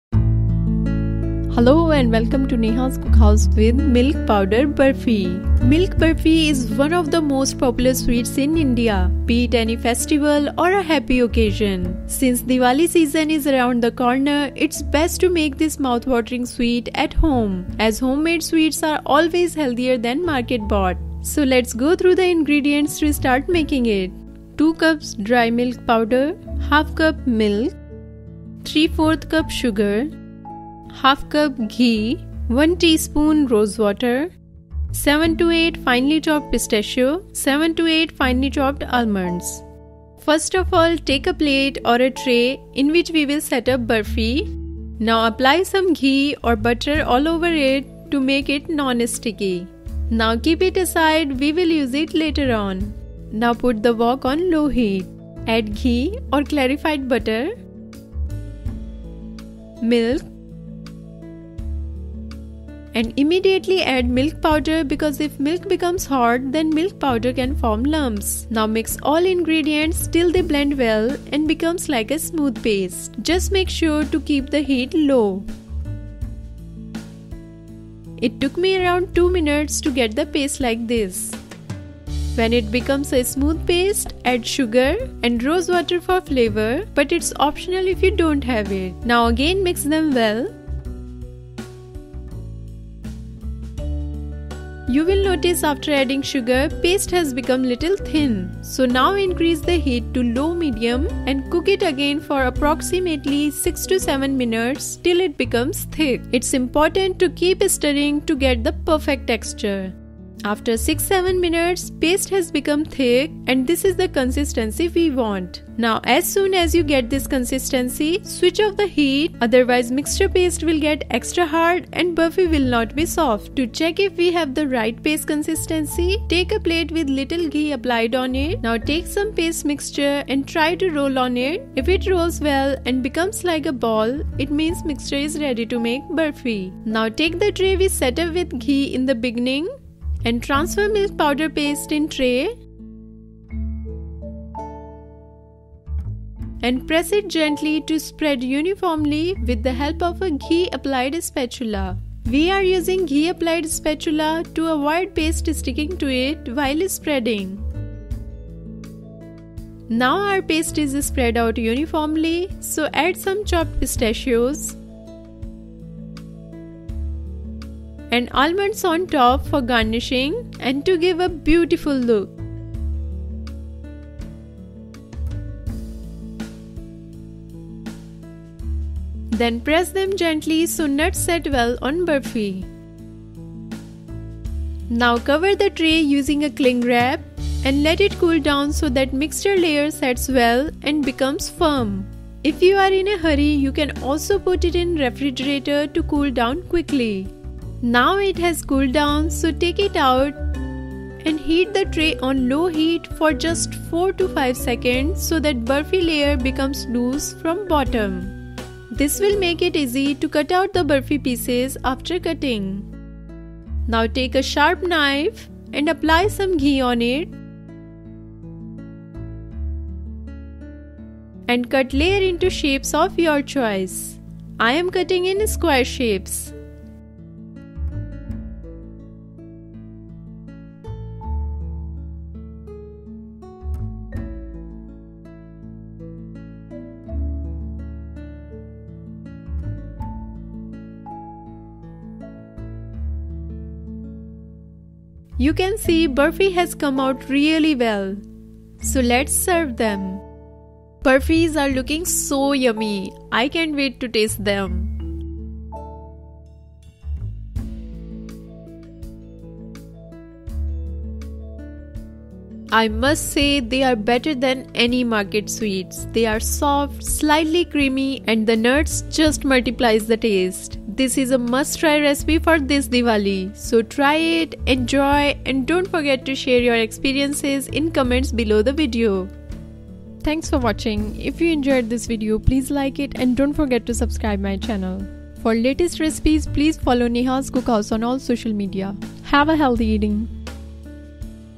Milk Powder Burfi Recipe | Easy Burfi Recipe | Diwali Sweets Recipe | How to Make Milk Powder Burfi
Milk Burfi is one of the most popular sweets in India, be it any festival or a happy occasion. Since Diwali season is around the corner its best to make this mouthwatering sweet at home, as homemade sweets are always healthier than market bought. These burfies are soft, slightly creamy and the nuts just multiplies the taste. This is a must try recipe for this Diwali. So, try it, enjoy and don’t forget to share your experiences in comments below the video.

Other Names:

Barfi, Milk Burfi, Milk Barfi, Milk Powder Burfi, Milk Powder Barfi, Condensed Milk Burfi

Ingredients:

2 cups Dry Milk Powder
½ cup Milk
¾ cup Sugar
½ cup Ghee (aka unsalted clarified butter)
1 tsp Rose Water (optional for flavor)
7-8 Pistachios (finely chopped)
7-8 Almonds (finely chopped)

Instructions:

1. First of all take a plate or tray in which we will set up burfi, now apply some ghee or butter all over it to make it non sticky. Keep it aside we will use it later.

2. Now put a wok on low heat, add ghee or clarified butter, milk and immediately add milk powder because if milk becomes hot than milk powder can form lumps. Now mix all ingredients, till they blend well and become like a smooth paste, just make sure to keep the heat low, it took me around 2 mins to get paste like shown in video.

3. When it becomes a smooth paste add sugar and rose water for flavor but its optional if you don’t have it, now again mix them well. You will notice after adding sugar, paste has become little thin, so now increase heat to low medium and cook it again for approximately 6-7 mins till it becomes thick, its important to keep stirring to get the perfect texture.

4. After 6-7 mins paste has become thick and this is the consistency we want. Now as soon as you get this consistency switch of the heat otherwise mixture paste will get extra hard and burfi will not be soft.

5. To check if we have right paste consistency, take a plate with little ghee applied on it. Now take some paste mixture and try to roll on it, if it rolls well and becomes like a ball it means mixture is ready to make burfi.

6. Now take the tray we setup with ghee in the beginning, and transfer milk powder paste in tray and press it gently to spread uniformly with the help of a ghee applied spatula. We are using ghee applied spatula to avoid paste sticking to it while spreading.

7. Now our paste is spread out uniformly, so add some chopped pistachios and almonds on top for garnishing and to give a beautiful look. Then press them gently so nuts set well on burfi.

8. Now cover the tray using a cling wrap and let it cool down so that mixture layer sets well and becomes firm. If you are in a hurry you can also put it in refrigerator to cool down quickly.

9. Now it has cooled down, so take it out and heat the tray on low heat for just 4-5 sec so that burfi layer becomes loose from bottom. This will make it easy to take out the burfi pieces after cutting. Now take a sharp knife and apply some ghee on it and cut layer into shapes of your choice, I am cutting in square shapes.

10. Once all burfies are cut they are ready to serve.

Tips n Tricks:

1. Make sure to immediately add milk powder without milk getting hot to avoid any lumps.
2. If you don’t have rose water you can also use other flavors of your choice.
3. You can also change nuts on top of burfi, according to your own choice.
4. If you want to cool burfi layer quickly, then put it into refrigerator for atleast a day.